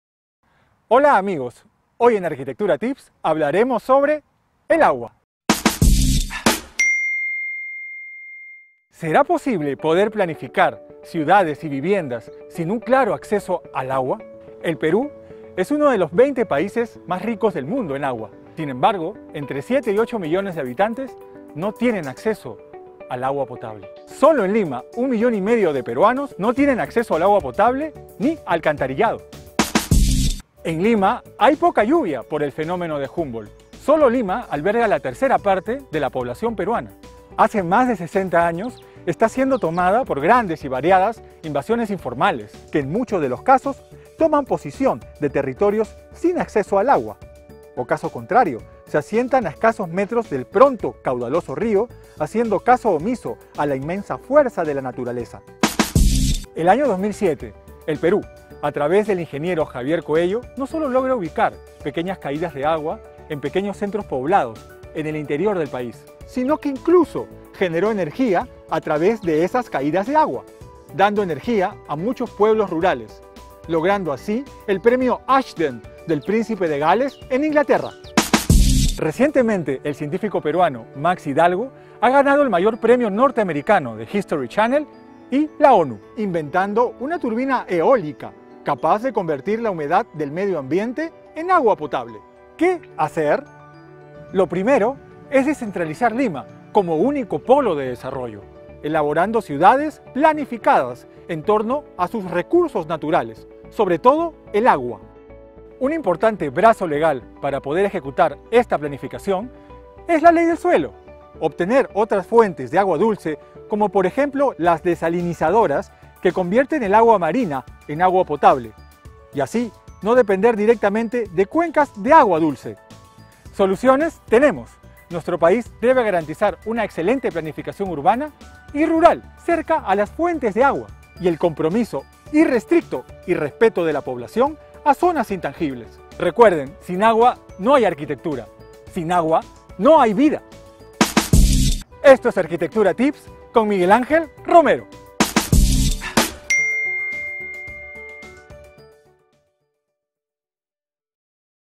Arquitectura tips: El agua
Tema: El agua
Conducción: Miguel Ángel Romero, arquitecto.